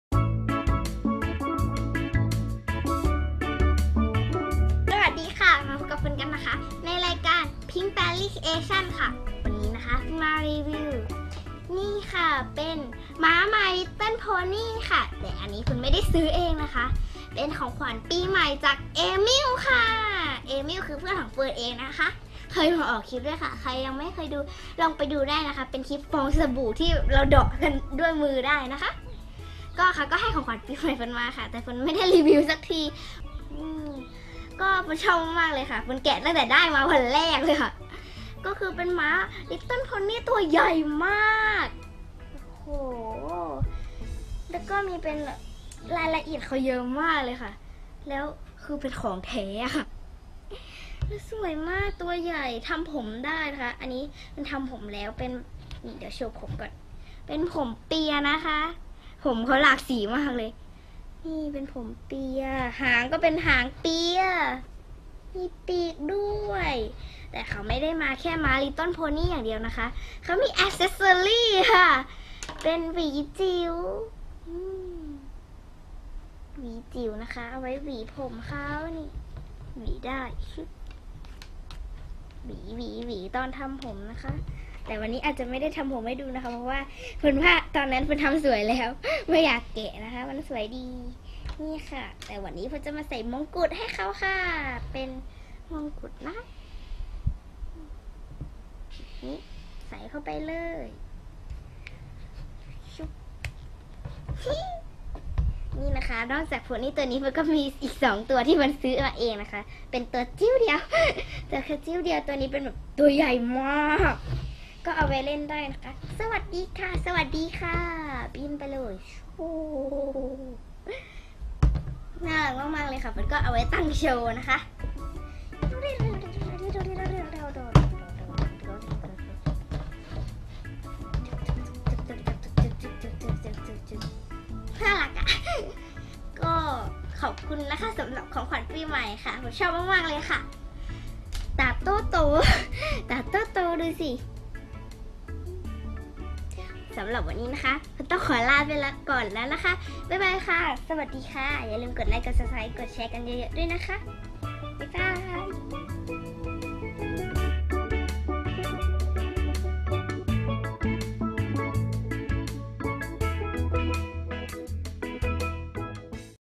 ม้าโพนี่ My Little Pony ของขวัญจากเอมิล | พิ้งค์ แฟรี่
พิ้งค์ แฟรี่ รีวิว - เฟิร์นได้ม้าโพนี่แสนน่ารักตัวนี้เป็นของขวัญปีใหม่จากเอมิล เพื่อนของเฟิร์นที่เคยมาถ่ายคลิปด้วยกัน ตั้งแต่ปีใหม่แล้วค่ะ เพิ่งได้เอามารีวิวให้เพื่อนๆได้ชมกัน